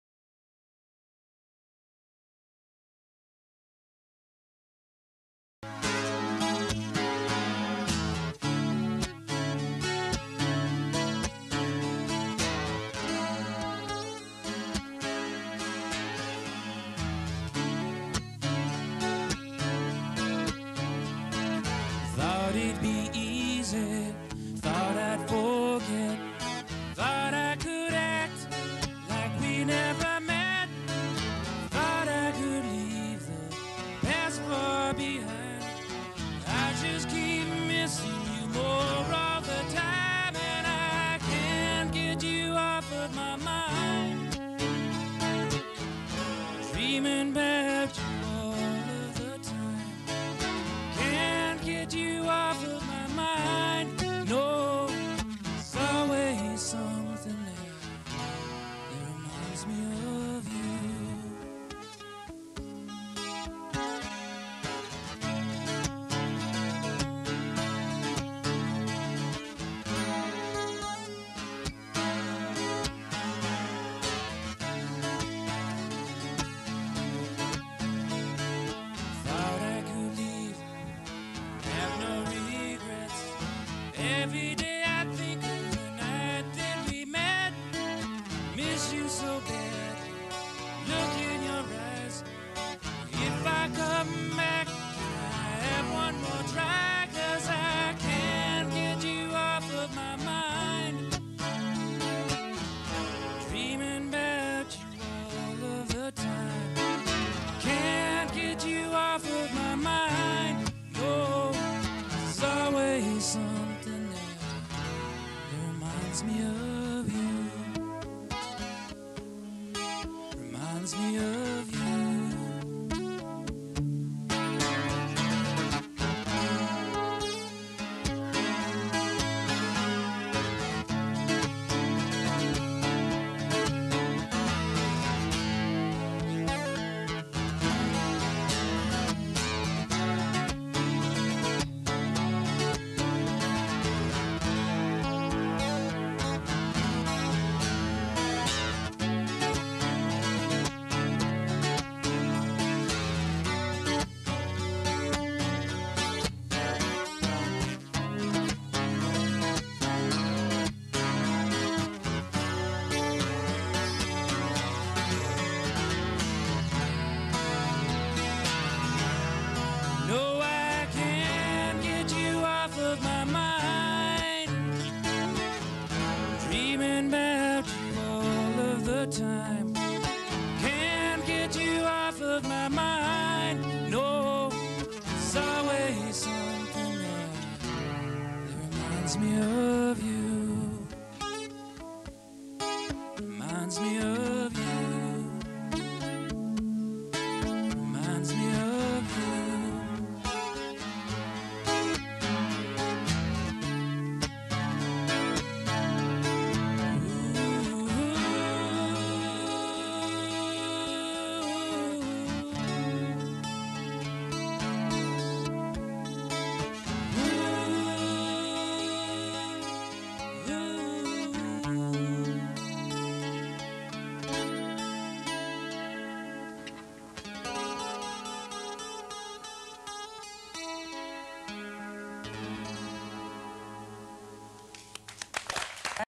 Steve Garcia -Acoustic music classic-Backstage In Arizona - Circa 1985
Here is someone I always enjoyed listening to -his relaxed and haunting melodies.